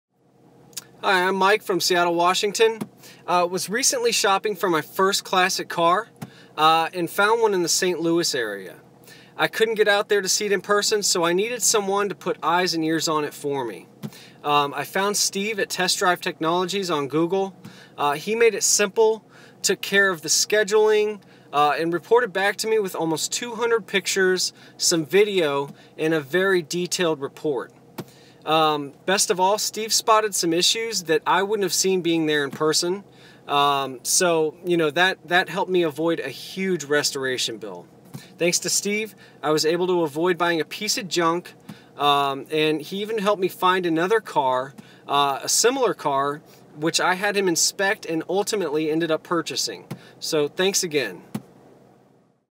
St Louis Classic Car Inspection Review from Customer in Seattle, WA
Mike from Seattle, WA sent us this video review after we inspected a classic car for him in St Louis... The first car we inspected he passed on due to some major corrosion issues we found.  We helped him locate another classic car and he purchased it.  Thanks for the great video review Mike! Get more info on our pre-purchase vehicle inspections